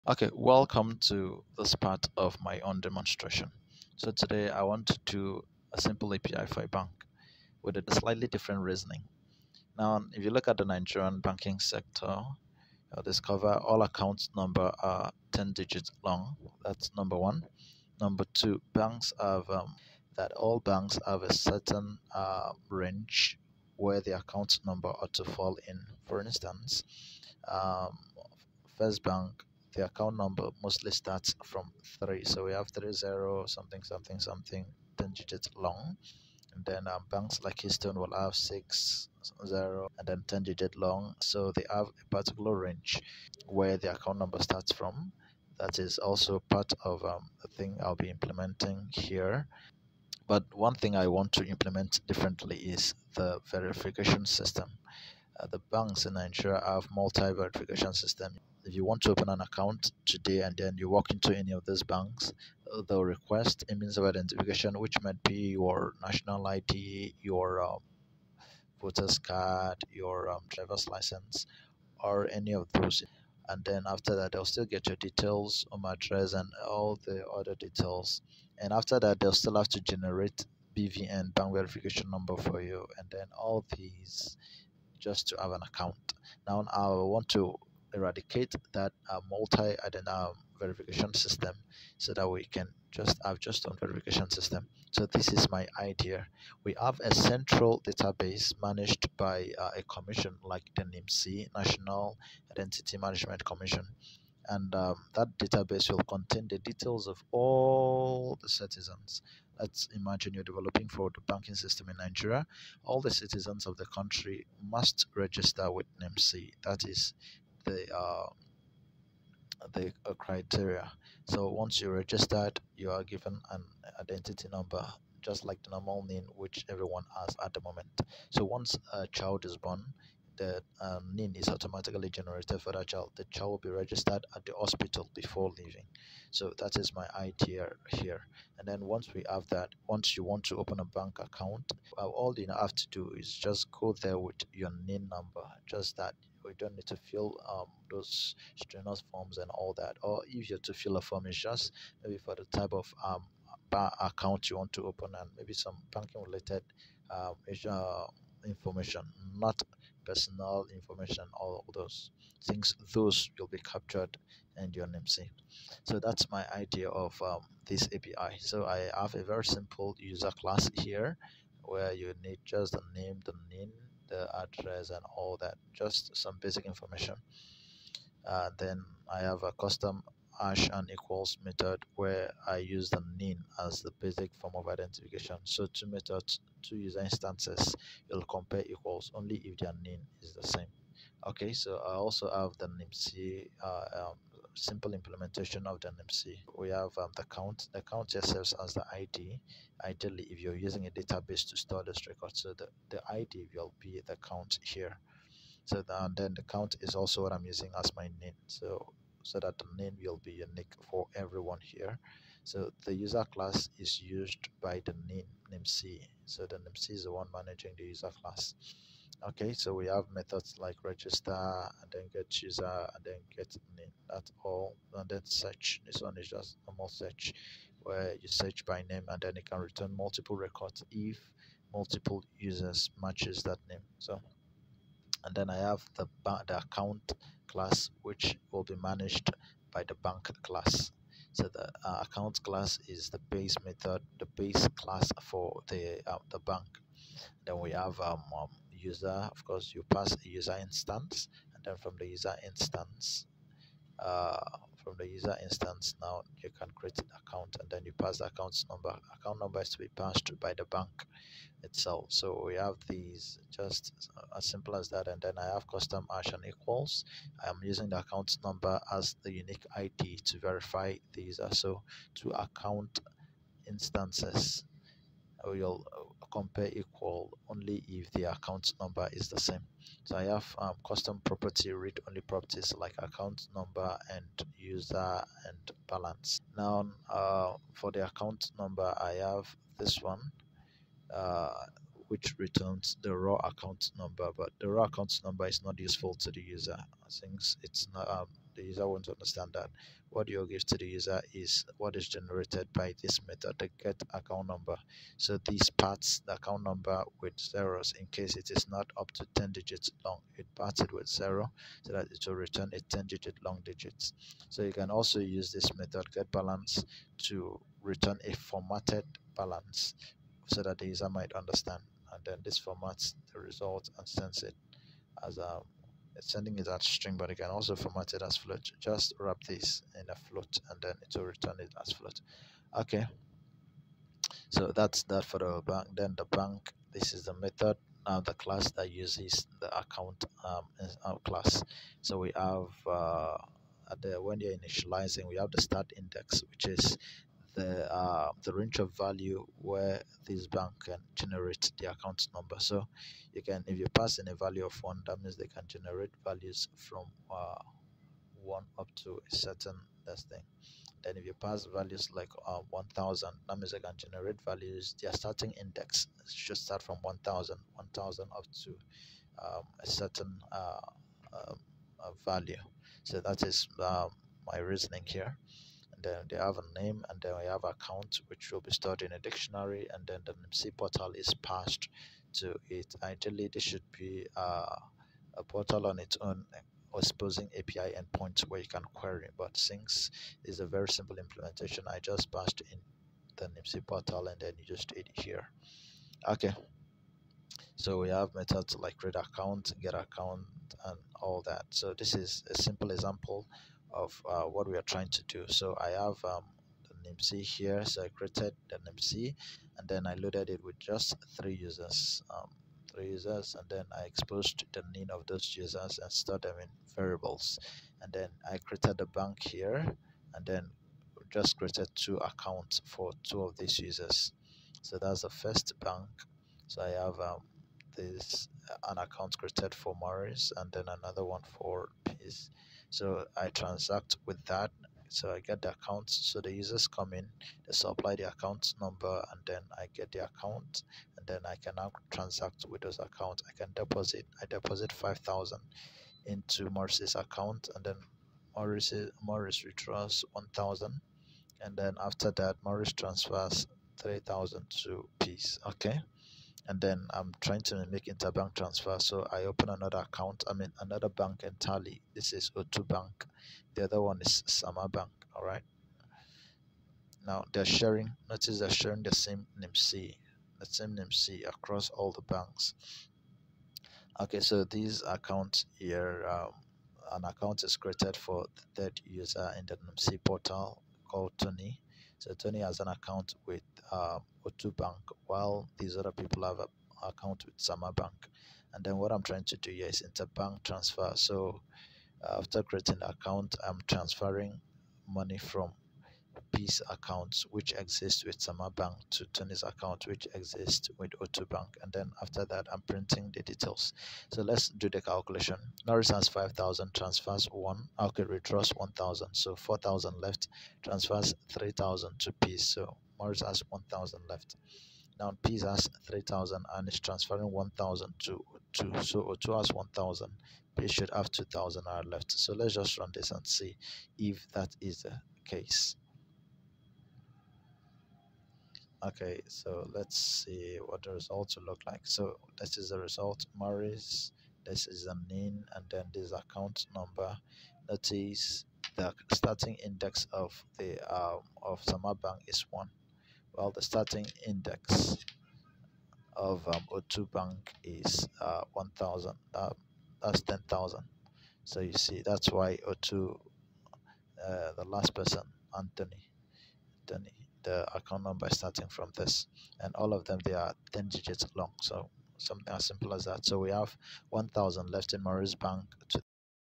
Bank API
Simple API for Banks that employs a slightly different reasoning. Here, only single identity method is employed where a central commission manages records of users and expose API endpoints for banks to use.

Customers only have to supply the identification number supplied by the commission to create bank accounts.